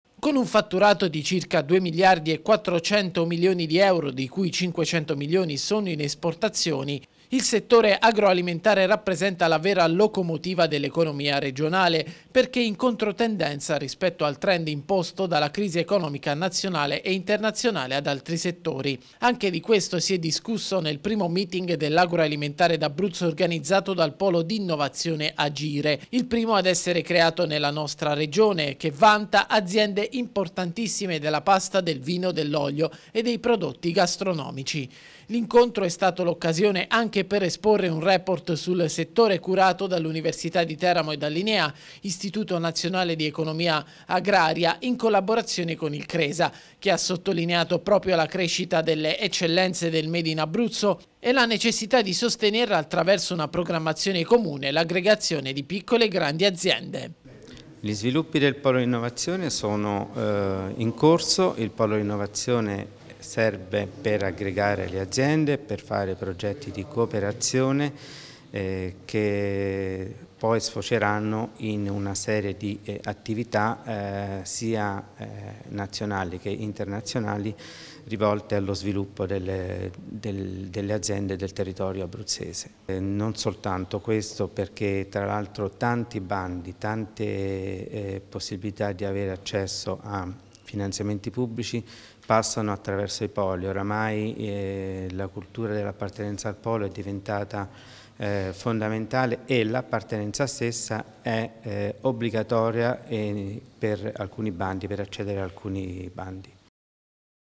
1° meeting dell'Agroalimentare
La crescita dell'agroalimentare d'Abruzzo e le iniziative per sostenerla al centro del 1° meeting dell'Agroalimentare